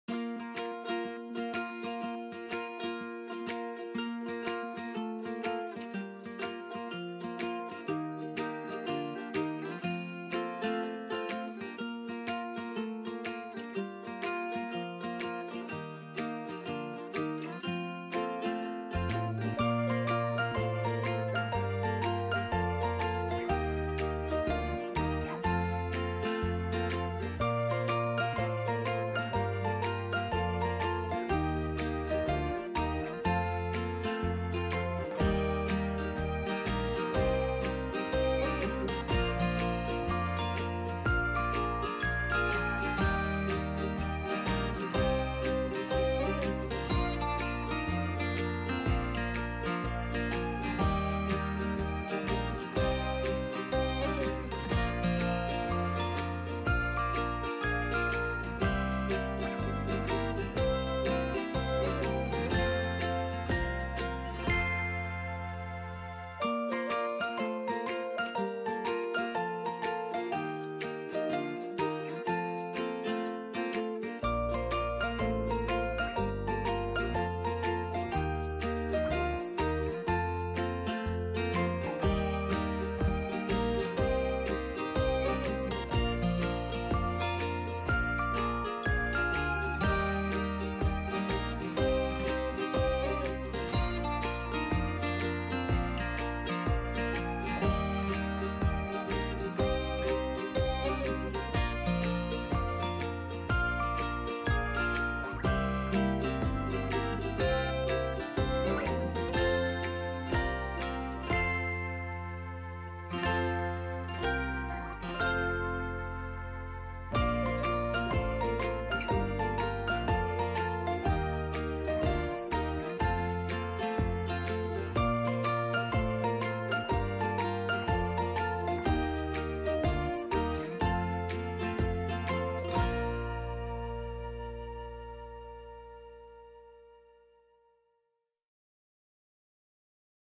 DIPTYQUE聖誕限定服務 香氛蠟燭藏自訂訊息
聖誕節係其中一個最浪漫嘅節日，有嘢想同女神講又唔好意思開口，不妨花啲小心思，Diptyque呢個聖誕特別服務，幫大家將心意收藏於香氛蠟燭內，點到大約一半時，對方就可以拿出字條，小編覺得好浪漫啊！

bmgproductionmusic